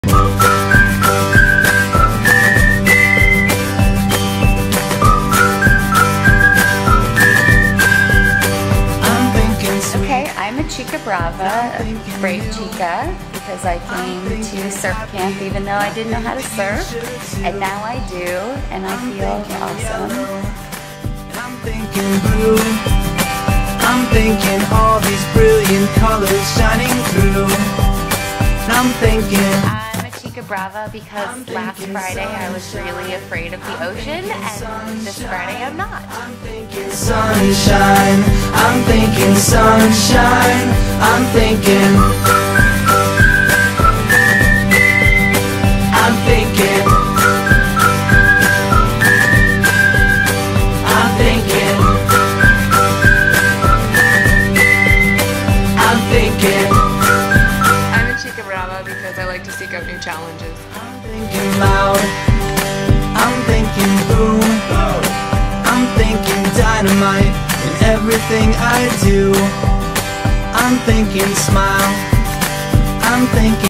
CHICABRAVA Nicaragua: Surf House June 6-13, 2015
This week the girls surfed, and ate tons of tacos! They also were able to surf in the bay. What a luxury to get out of bed and walk to the beach! Check out what makes them CHICABRAVAs!